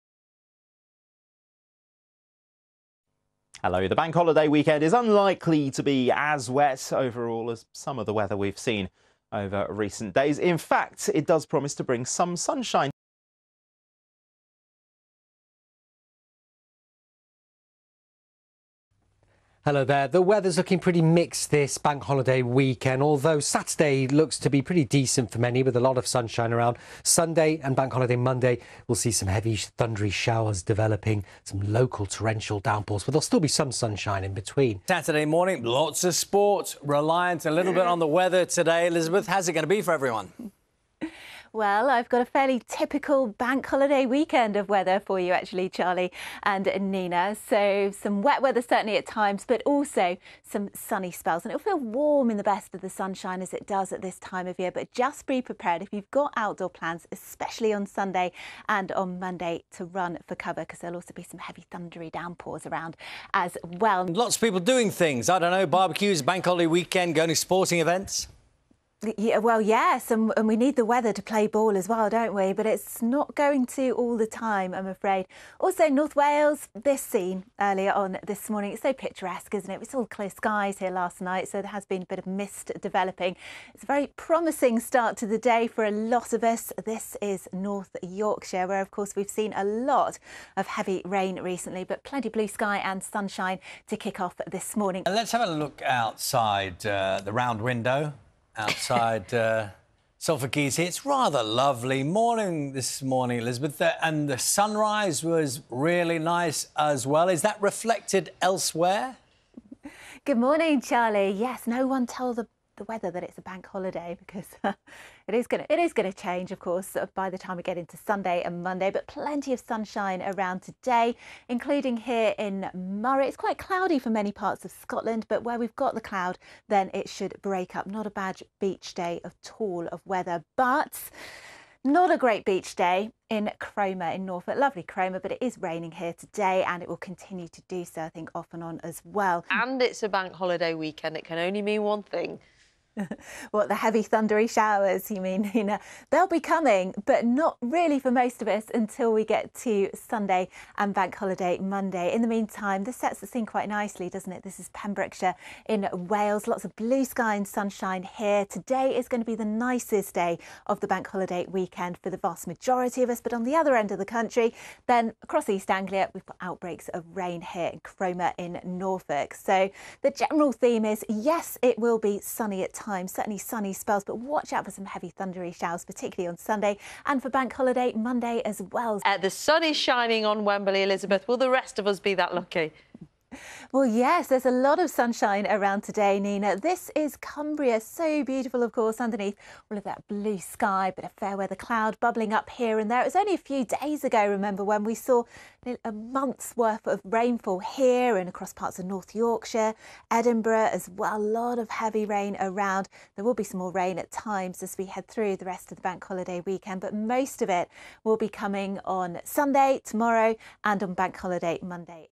Weather images + Bank Holiday weekend kicks off with the sun but not for long (UK) 25/May/2024 (a)
1) Listed and unlisted (library) weather images of the day (UK). If any.
2) Any outlooks and charts, etc. (if any).

By morning, (this is not a forecast), this is for weather images.